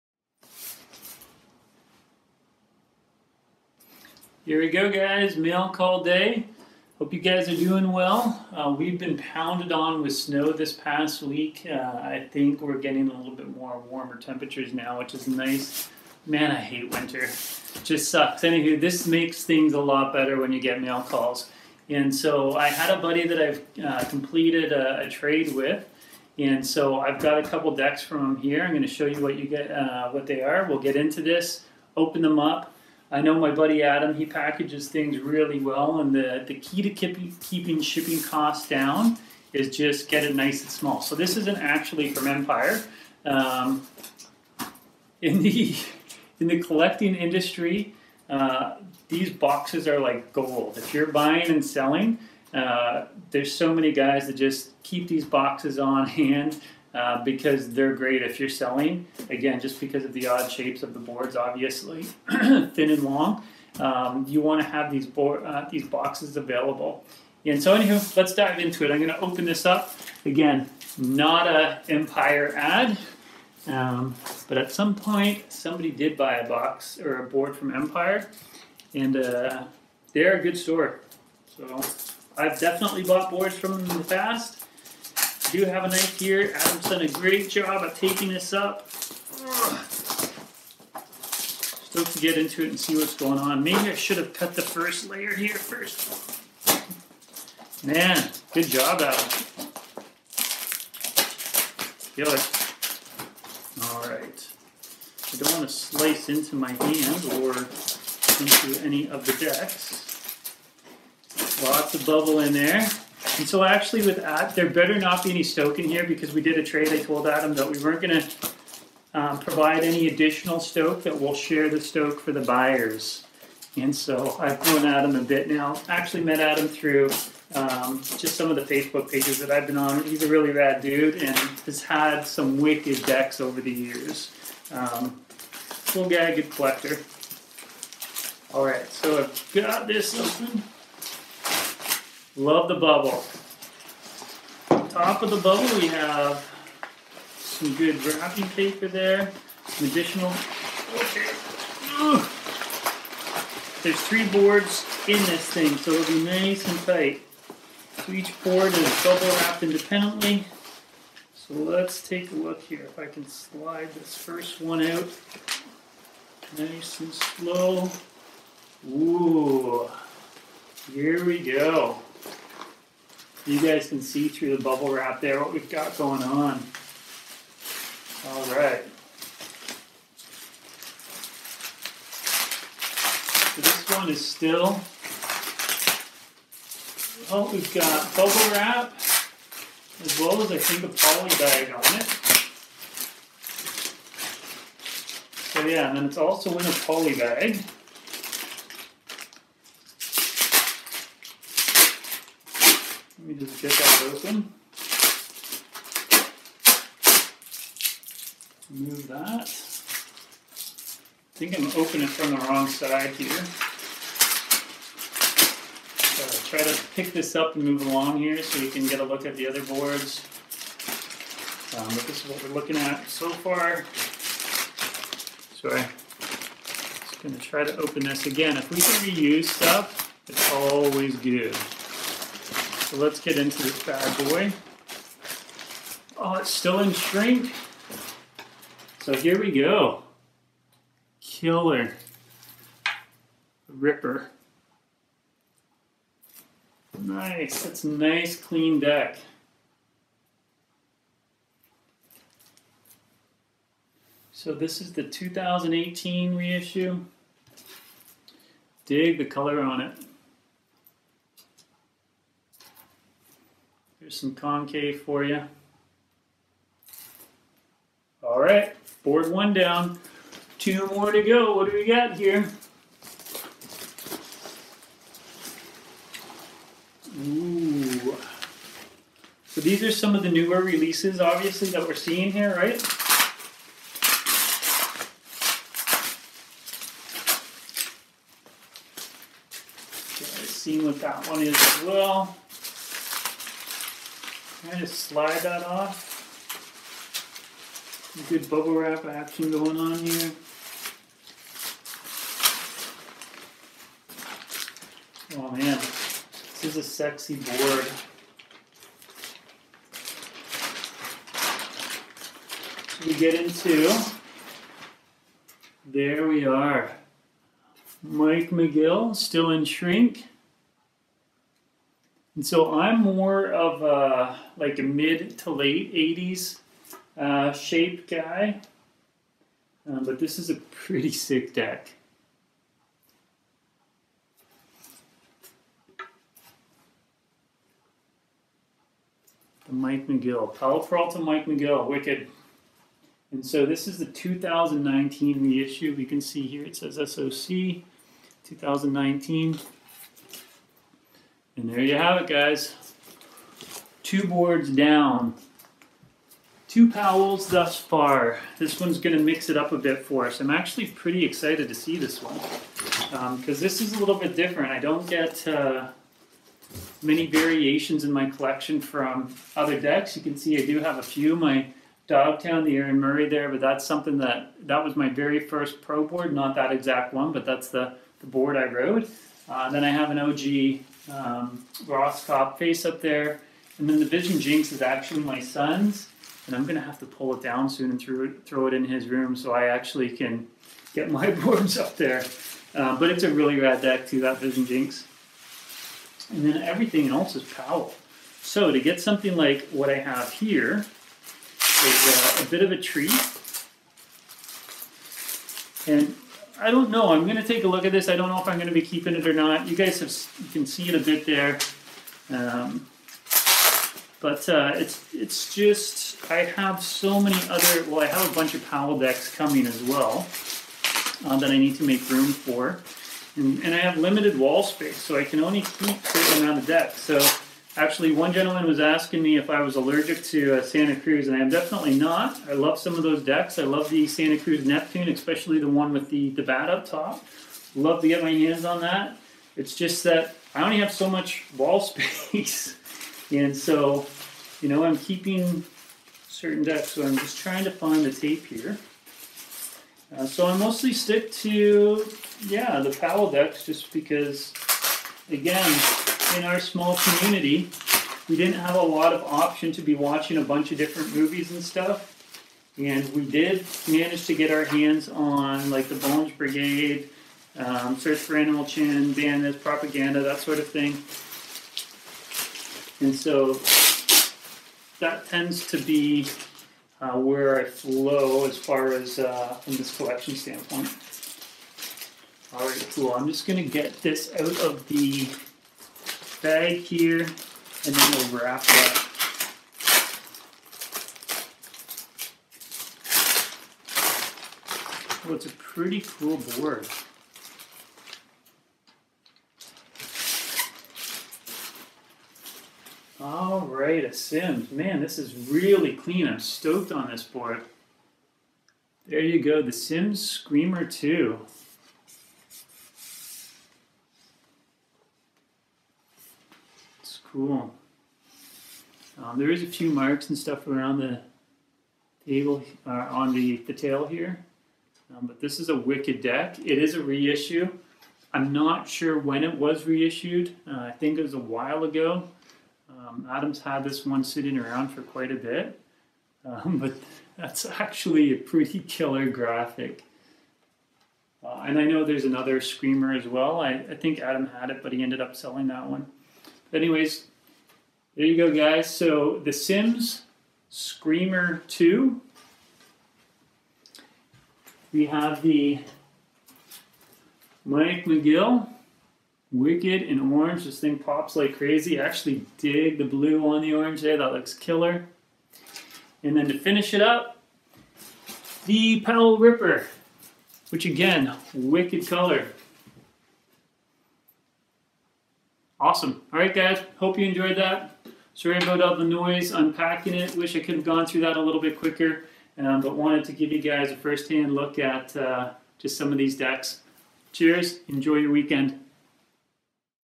Re-Issue Skateboard Deck Unboxing: Powell-Peralta and Sims Skateboards
Reissue skateboard deck unboxing of a trade I did with another Collector. Included is a Powell-Peralta Mike McGill, Powell-Peralta Ripper and a Sims Screamer 2.